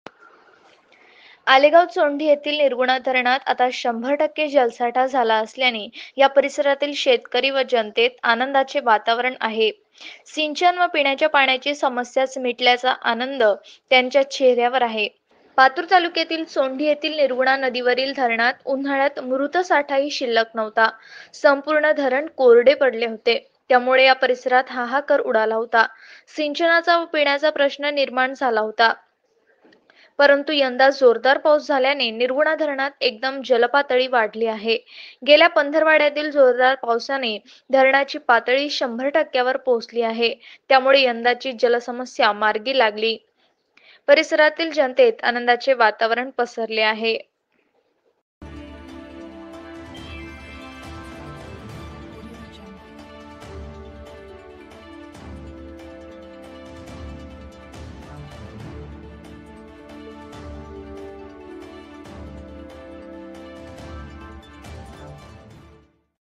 अकोला - निर्गुणा धरणात शंभरटक्के जलसाठा...
आलेगाव चोंढी परिसरात सिंचनाचा व पिण्याचा पाण्याचा प्रश्न मिटल्याने शेतकरी आनंदात